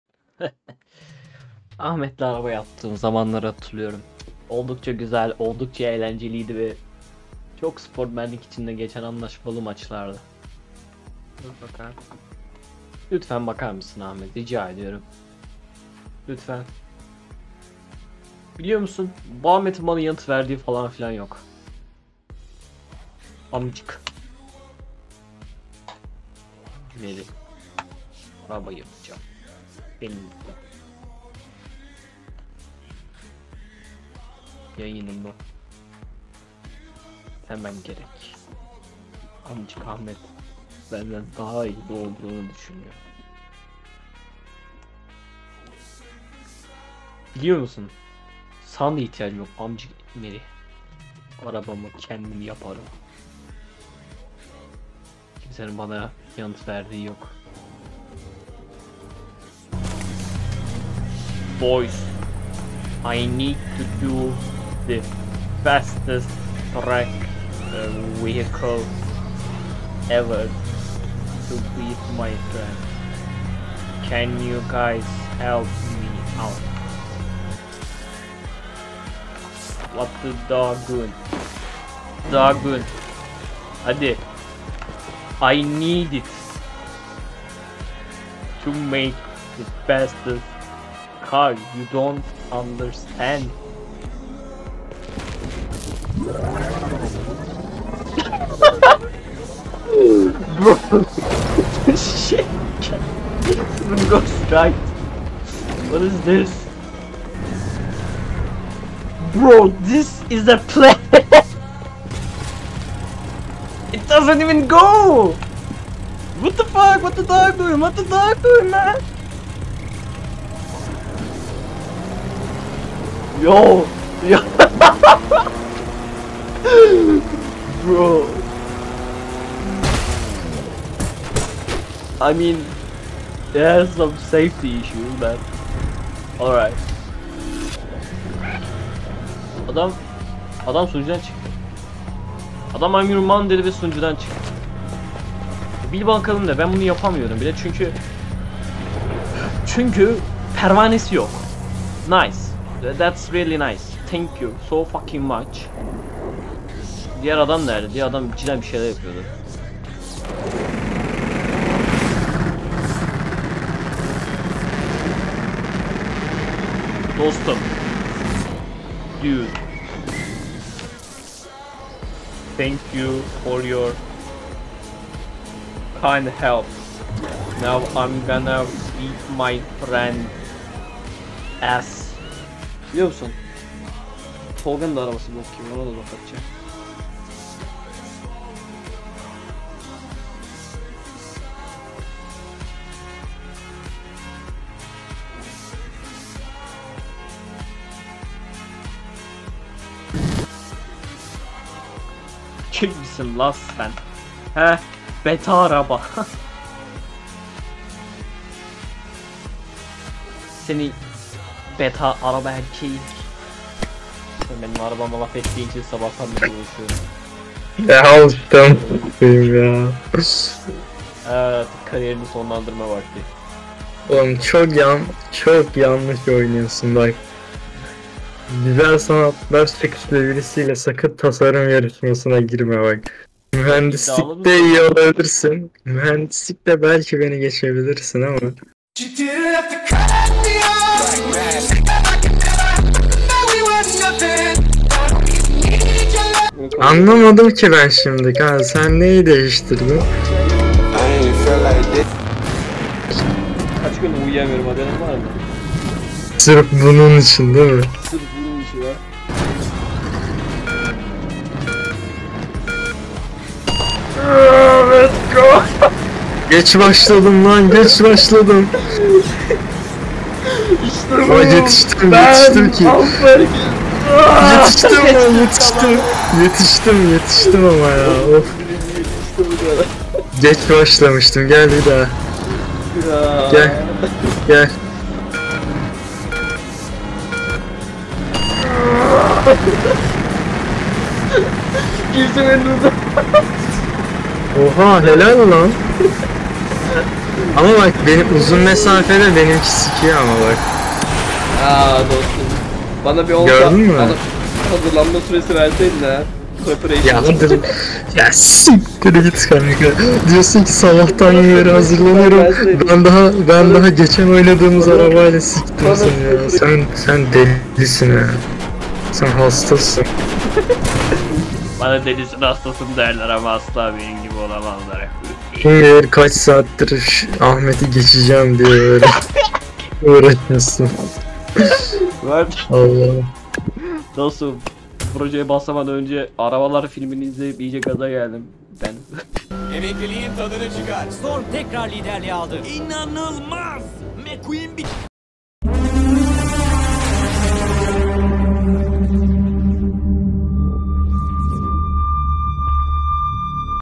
Arkadaşım (?) arabamı beğenmediği için (büyük hata) yeni bir araba yapıyorum (kolaydı)
Ahmet arabasına 32 tane Block 52 jet motoru yerleştirdiğinde rakibine döndü ve şöyle sordu:

En kötü araba sende mi olduğu için en kötü mekaniksin yoksa en kötü mekanik olduğun için mi en kötü araba sende?

Mekanik, Alan Genişletmesini ve yasaklı tekniklerden, kamera dışı inşa ve aerodinamiği kullanmadan önce Ahmet tekrar sordu:

Beni geçebilir misin peki?

Mekanik şöyle yanıtladı:

32 tane Block 52 jet motoruna sahip olan araban (?) eğer benden önce kalkış yaparsa başım biraz derde girebilir.

+Peki kaybeder miydin?

-Yok ya, kazanırdım.